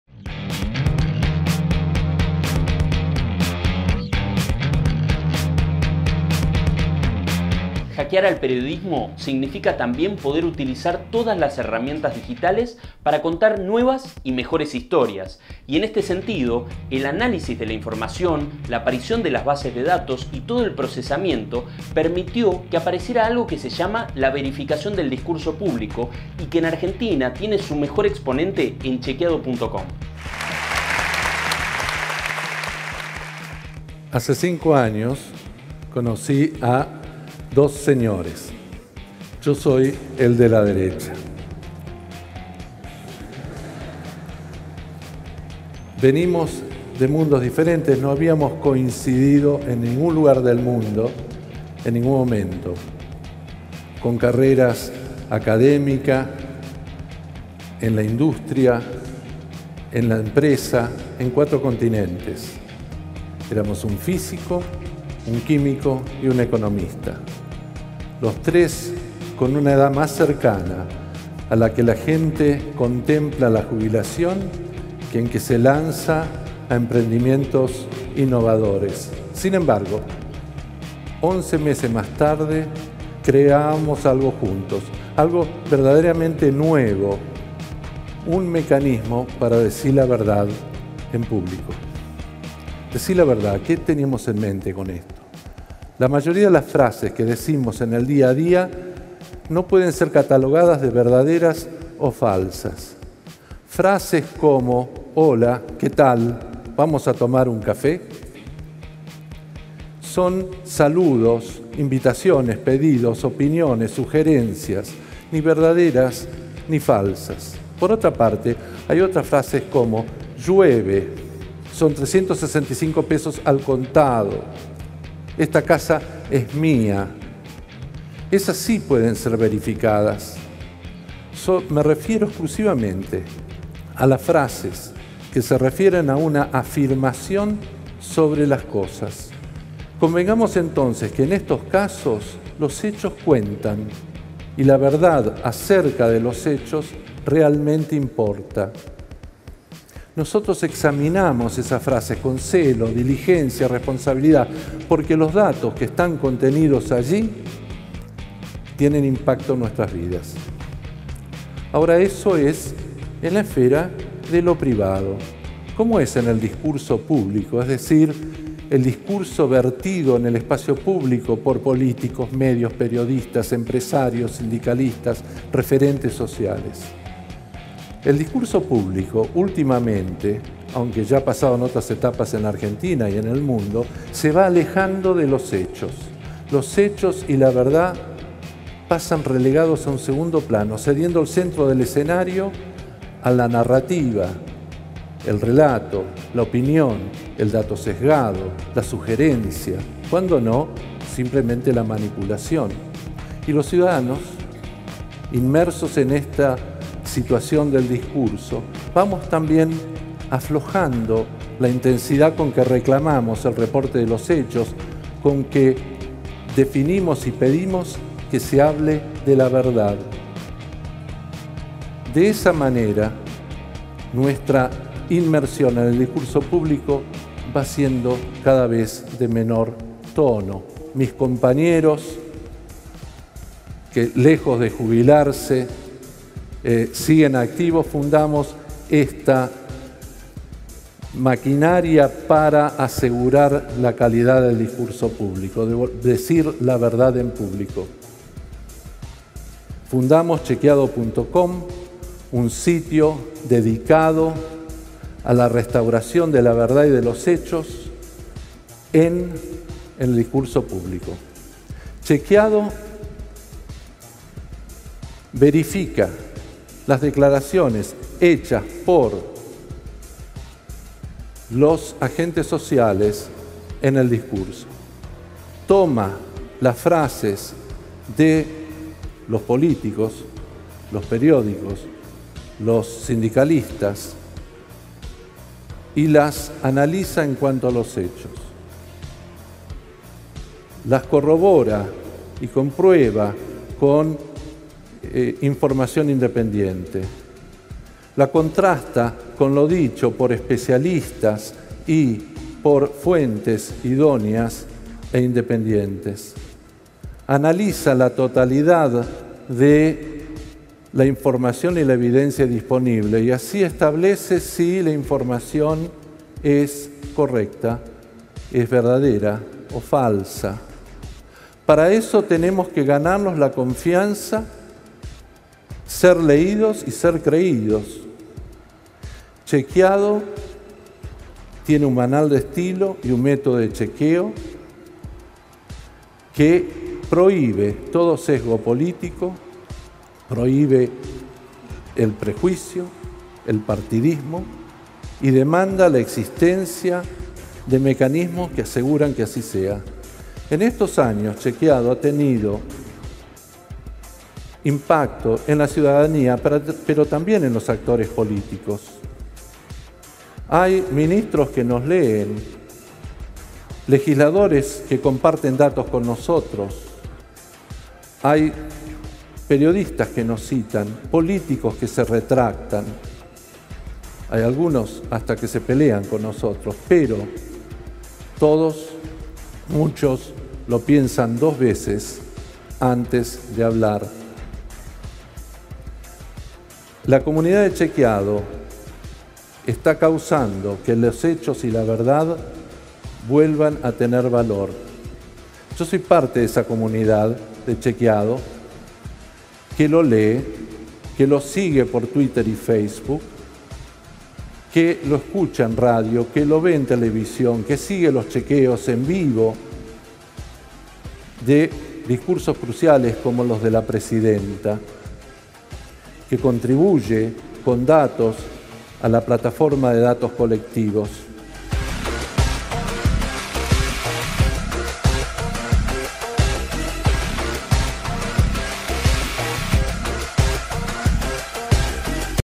La historia y filosofía de Chequeado.com
Ningún área del desarrollo humano quedó exento del avance de la tecnología. Pero el periodismo sufrió una de las transformaciones más llamativas, trasladándose de los textos de algunos profesionales en sus oficinas, a los teléfonos de los ciudadanos que quieran compartir información en Internet.